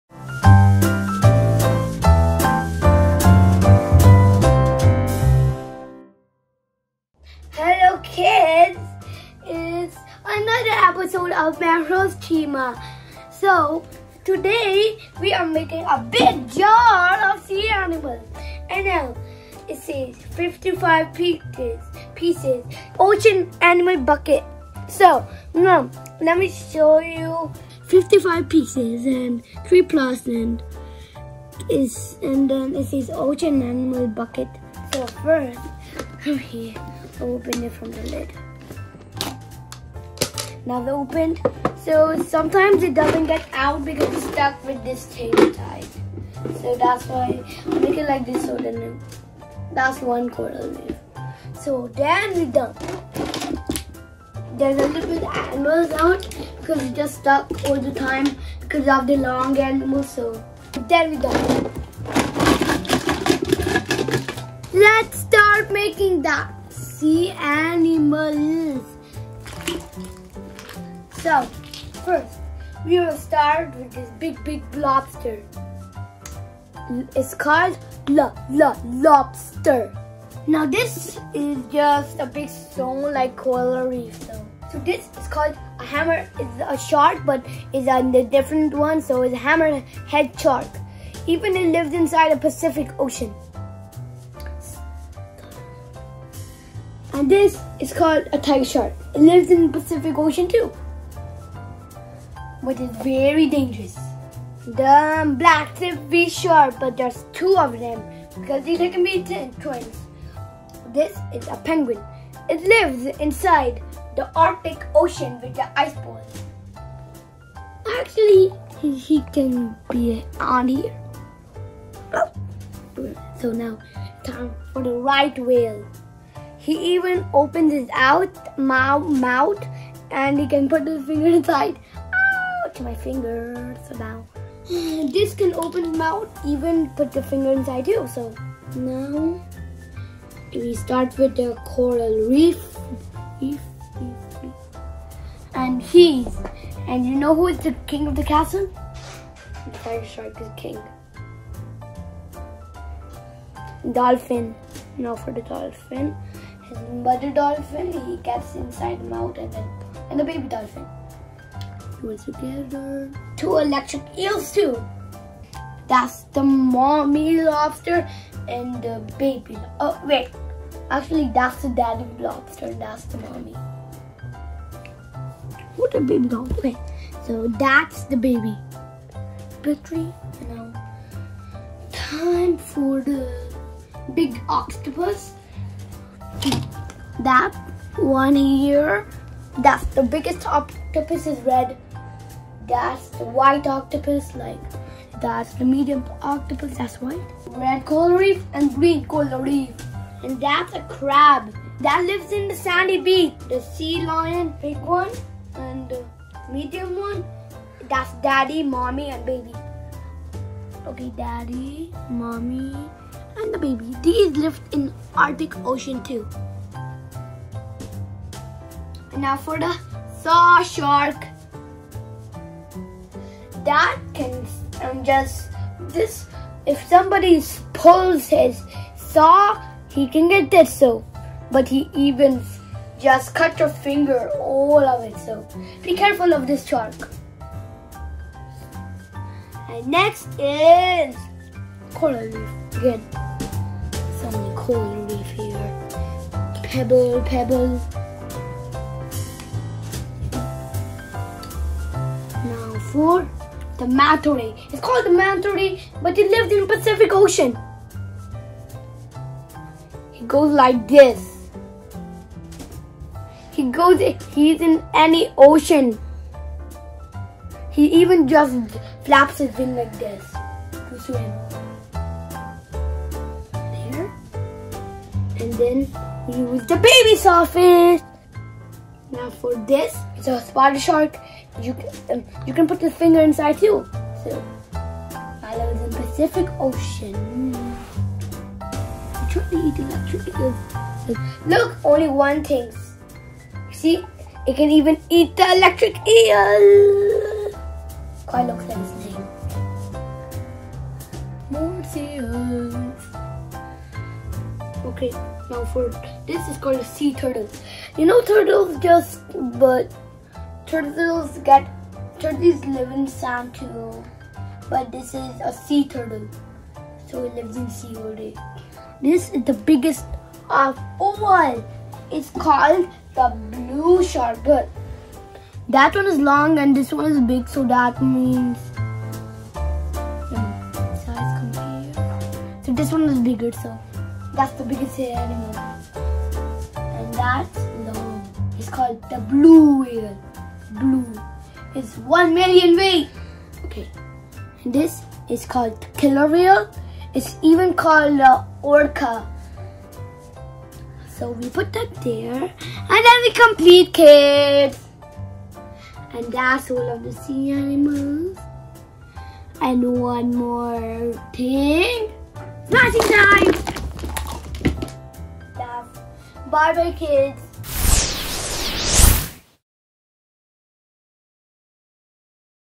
ocean animals kids toys review...
How you can play Ocean or Sea Animals toys Mehroz will unbox!!!